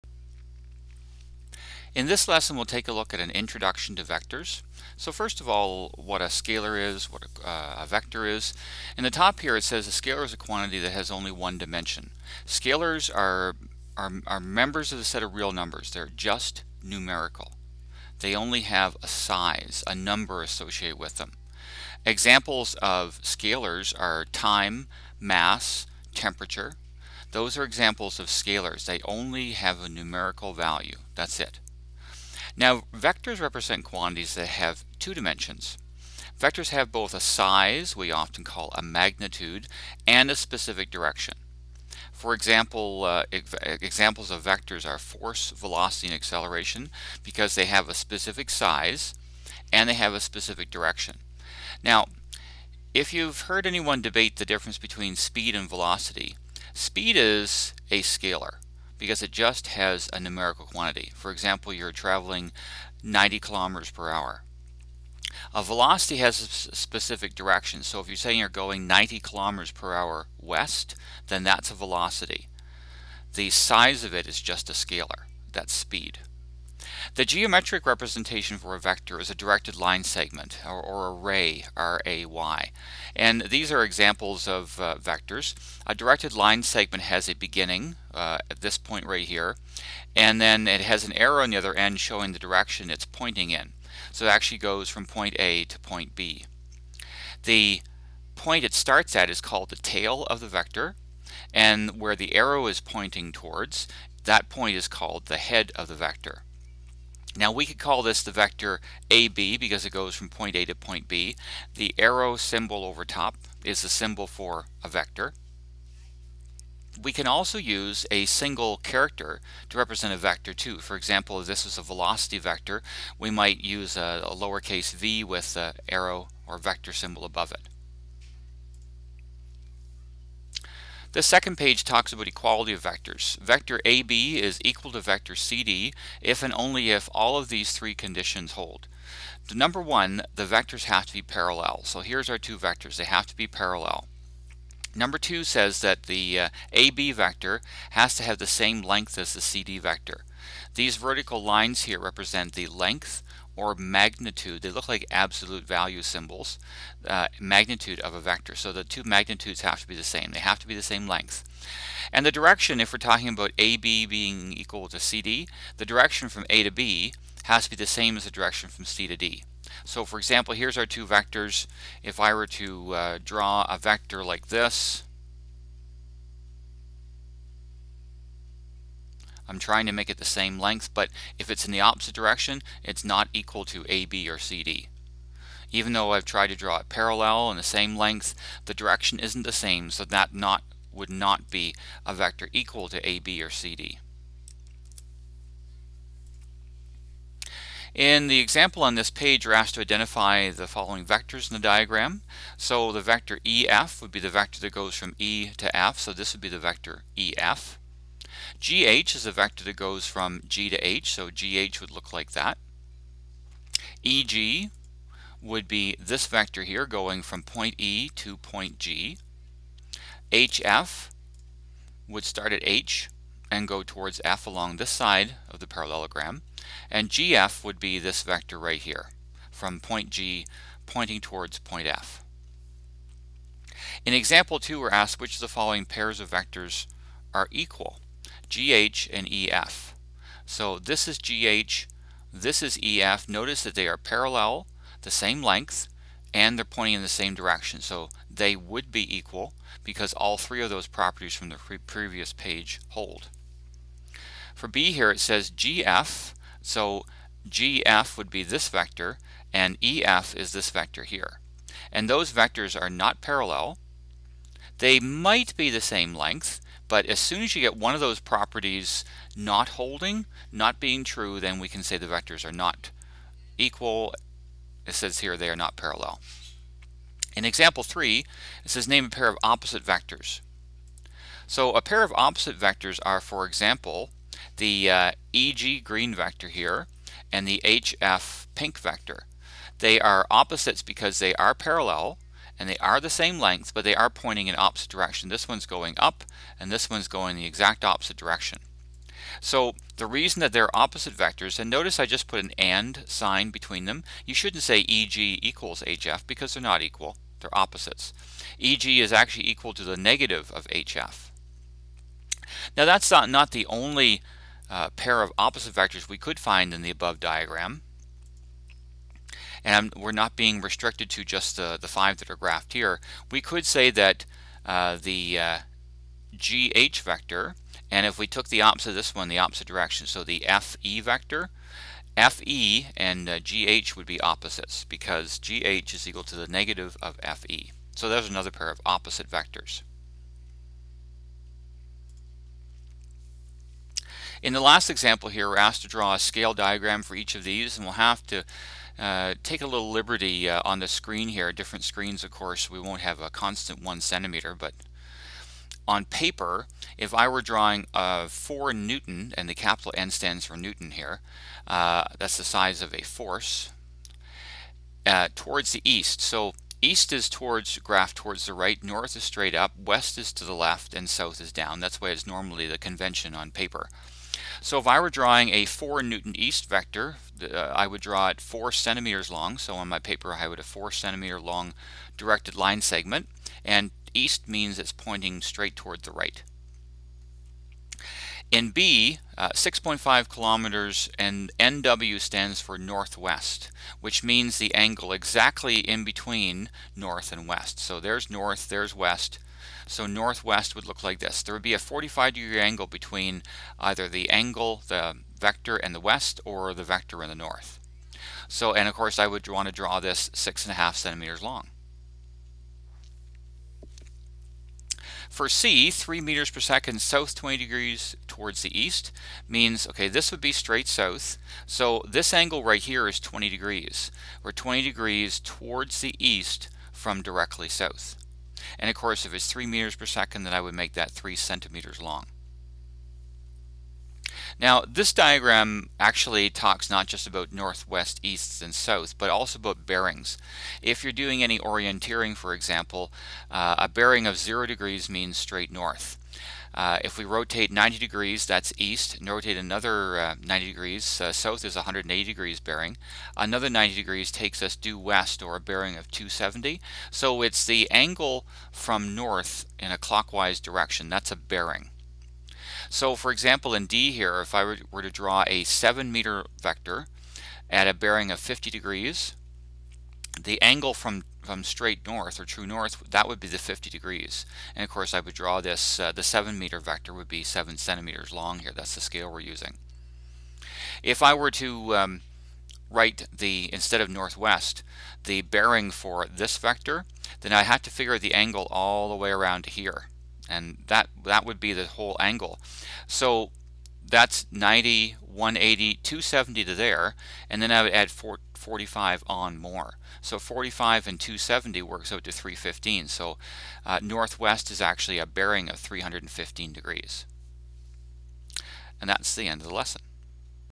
Introduction to Vectors
This lesson discusses the difference between scalars and vectors, equality of vectors and drawing geometric vectors.  This lesson was created for the Calculus and Vectors (MCV4U) course in the province of Ontario, Canada.